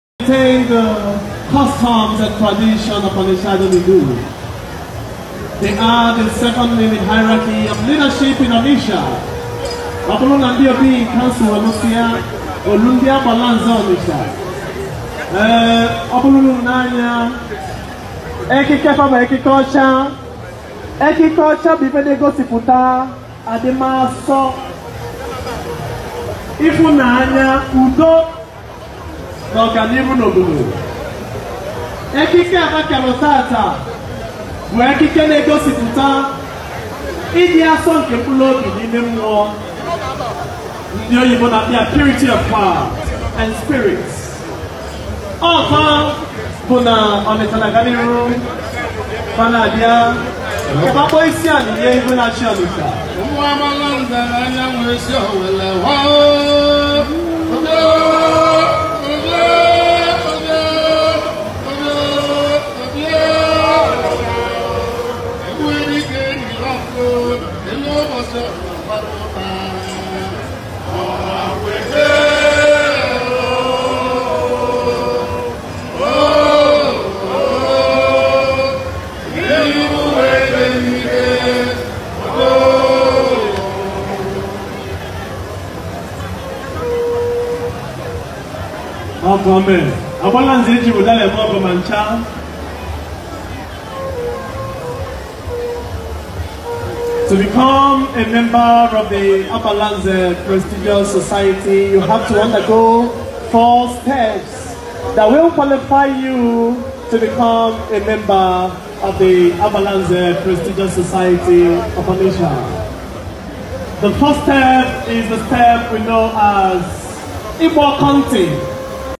Ofala HRM Obi Nnaemeka Alfred Achebe 11 October, 2019
Members of Prestiges Agbalanze Society of Onitsha Greeting King of Onitsha at Ofala festival, 11 October, 2019